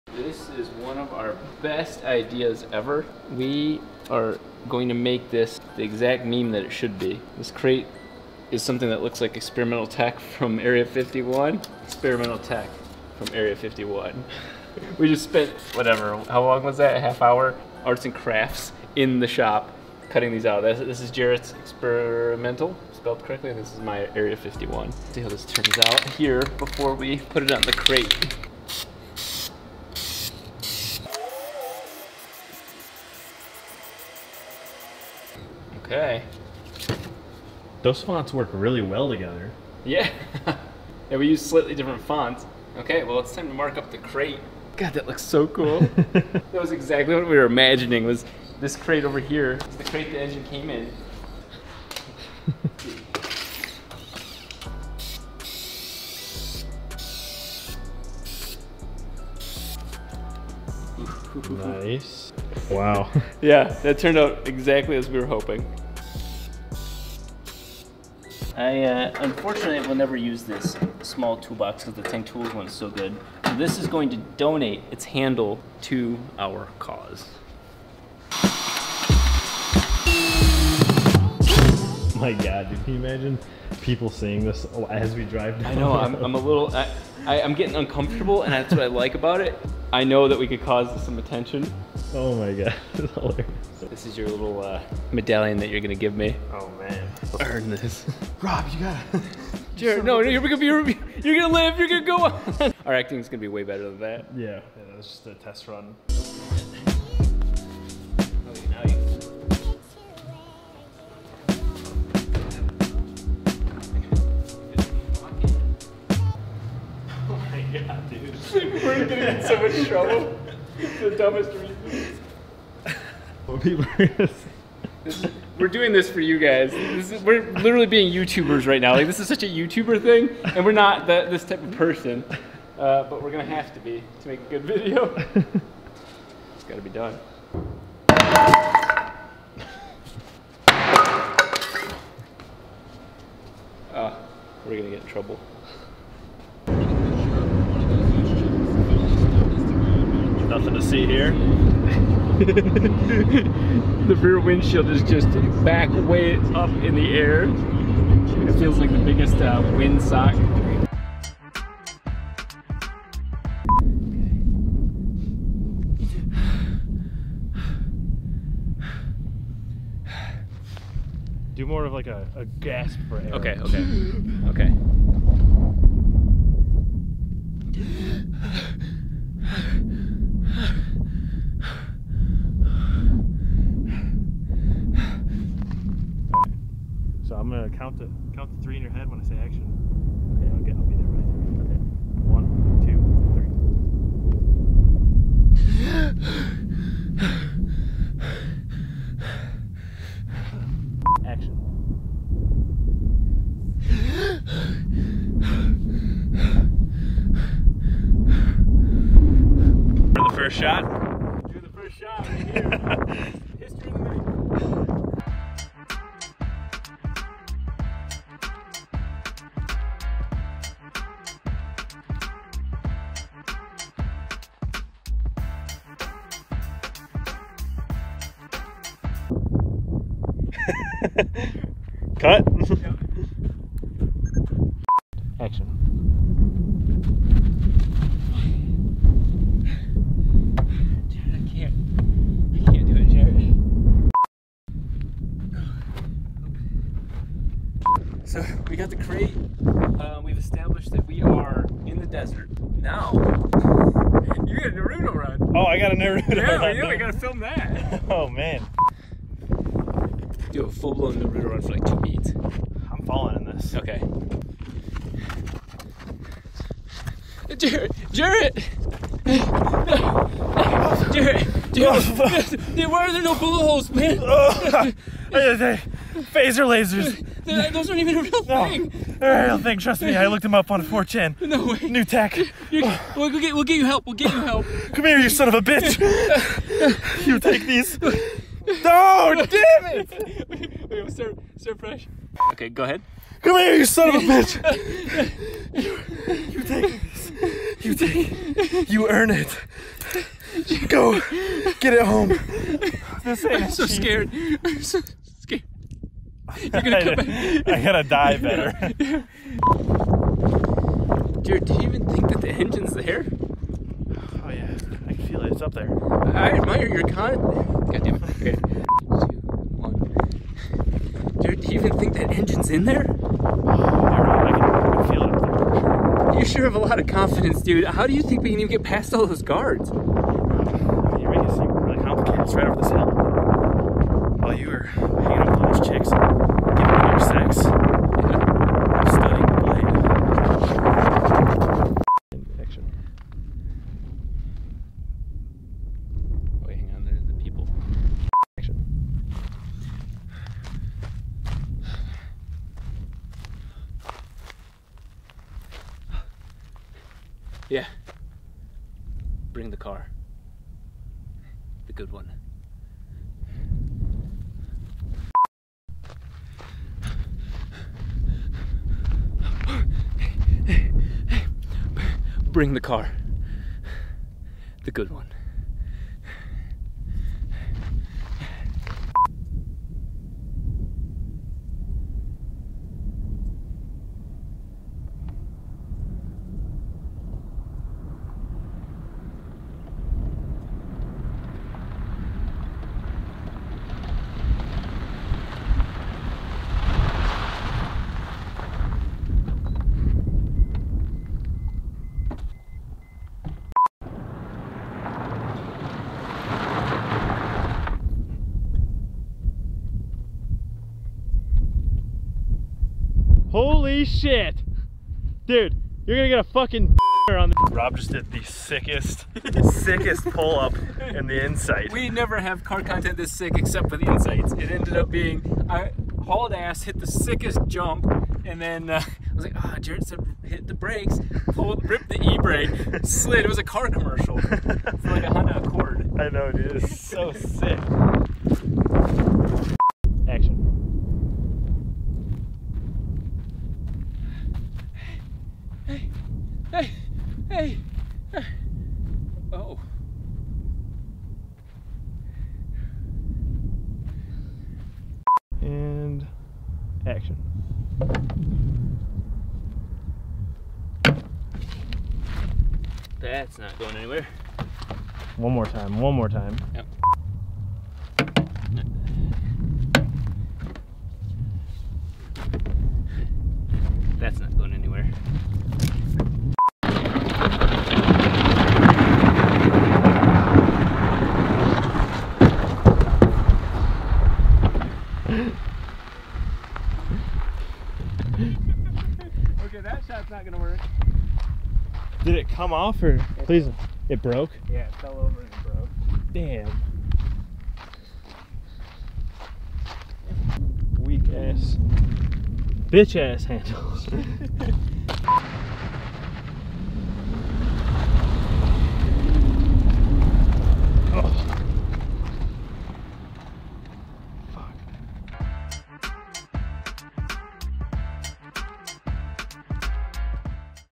Behind the scenes filming our Area 51 Crate video
Figured it would be fun to show the adventures involved with filming something more cinematic.